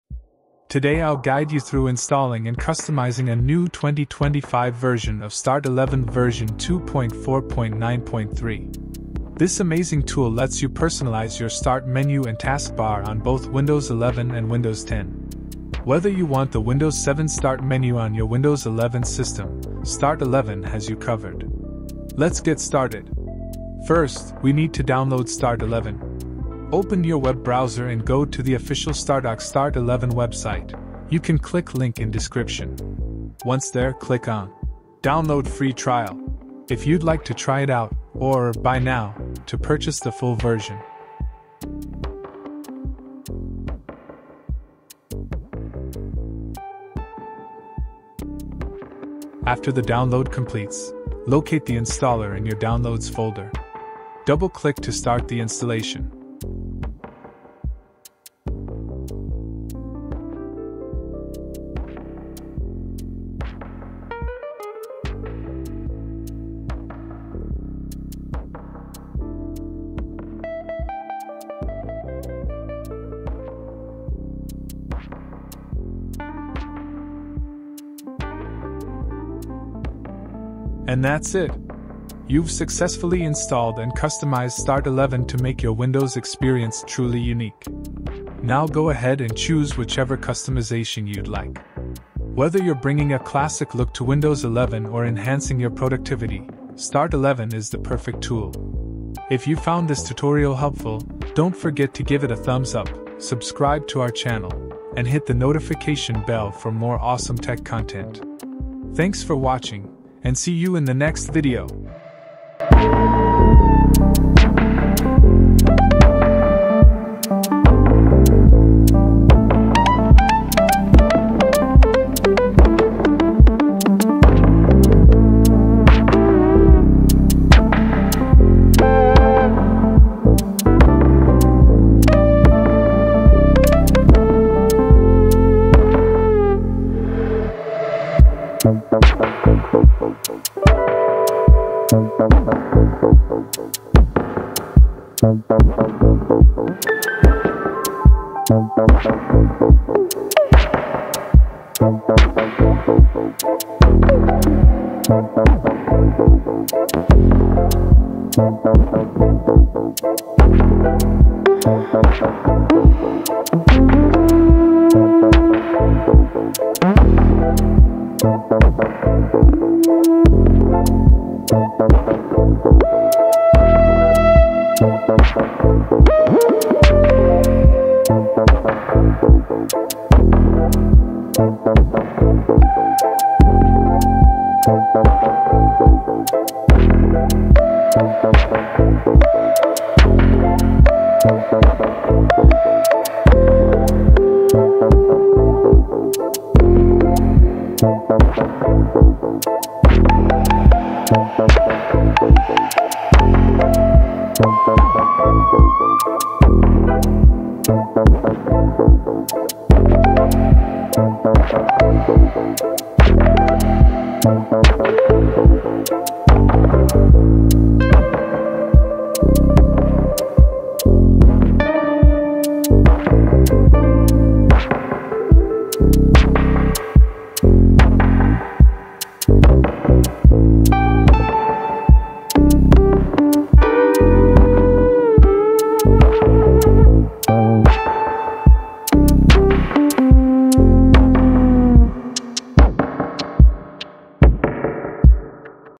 The All New 2025 Start11 Installation & Customization for Windows 11 & 10
Customize your Start Menu and Taskbar to suit your style, whether on Windows 10 or Windows 11.